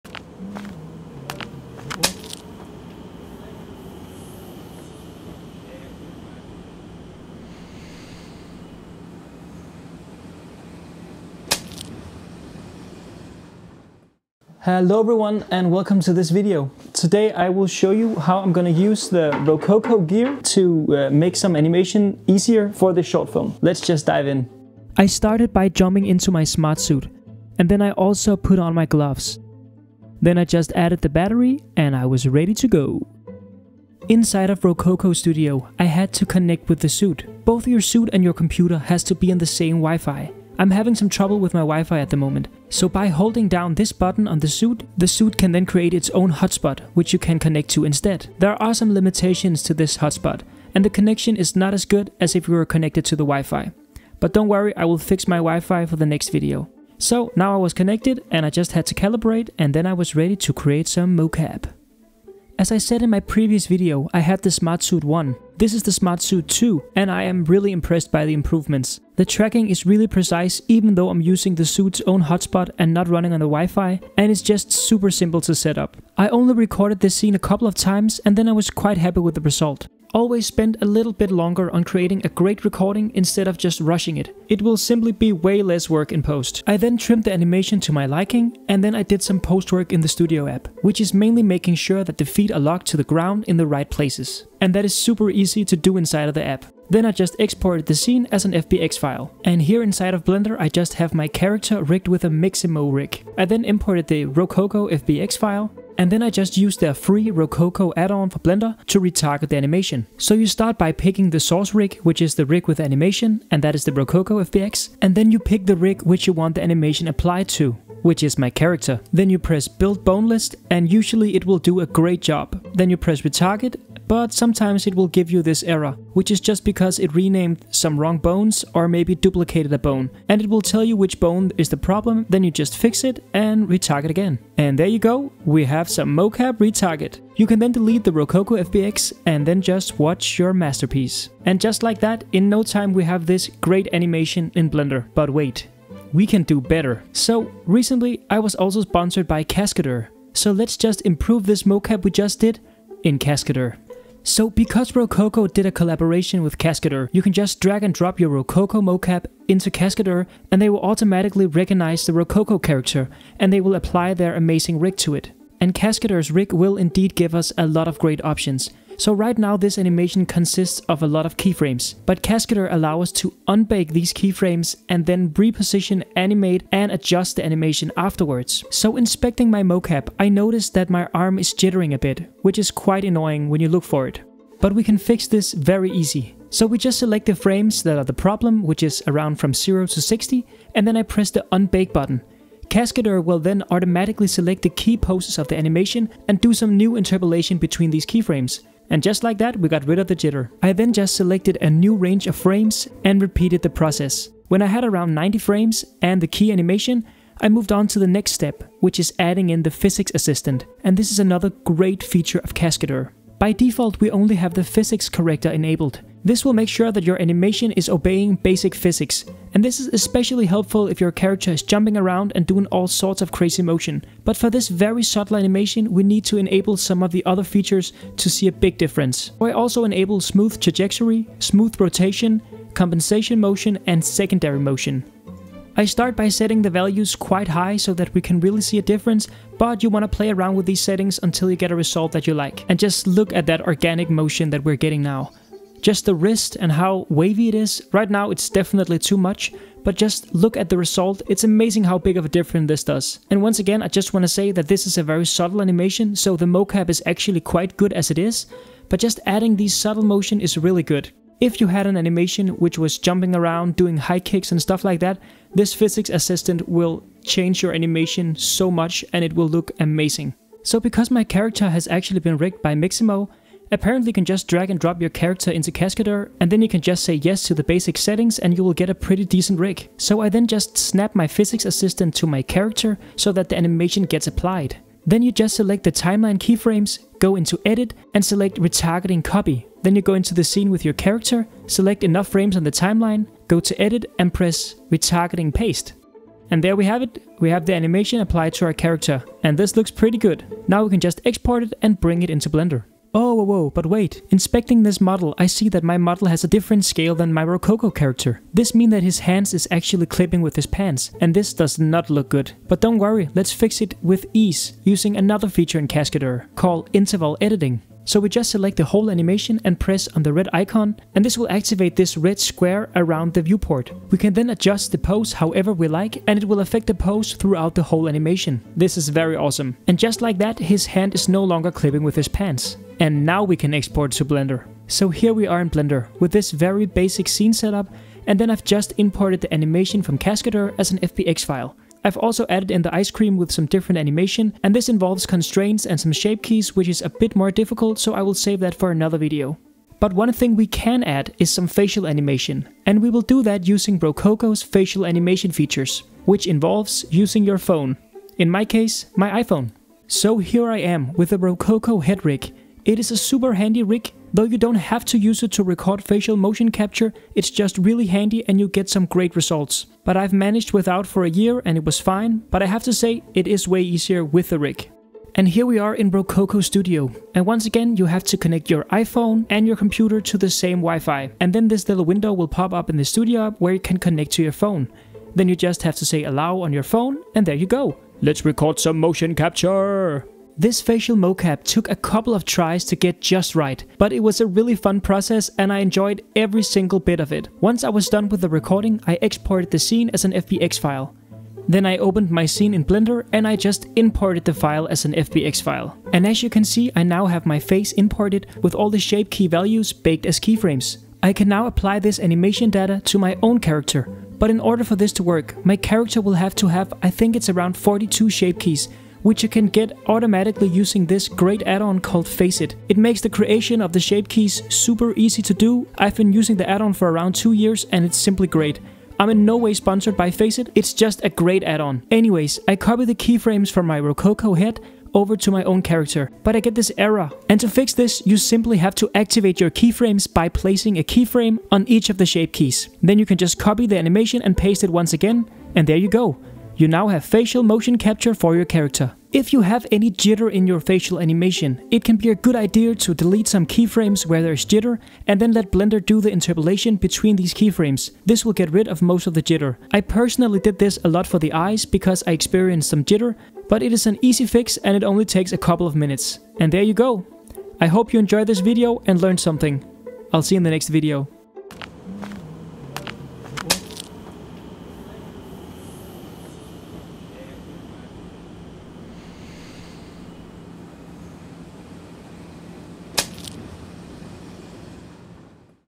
Easy High-Quality Animation in Blender with Rokoko & Cascadeur (Step-by-Step Tutorial)
Learn how to create professional-quality animation in Blender easily! In this tutorial, I’ll show you how to combine Rokoko's motion capture with Cascadeur's AI-assisted animation tools to quickly achieve smooth, lifelike character movement—perfect for games, films, and more.

✅ What you’ll learn:

Setting up Rokoko motion capture

Enhancing animation with Cascadeur

Importing into Blender for final polish

Tips for clean, realistic movement

Use my Discount: 5 procent off all orders: RKK_ANIMATABLE

Add-ons you should have for Blender!

Another way to support my journey!

"Animatable breathes life into imagination—one frame at a time!"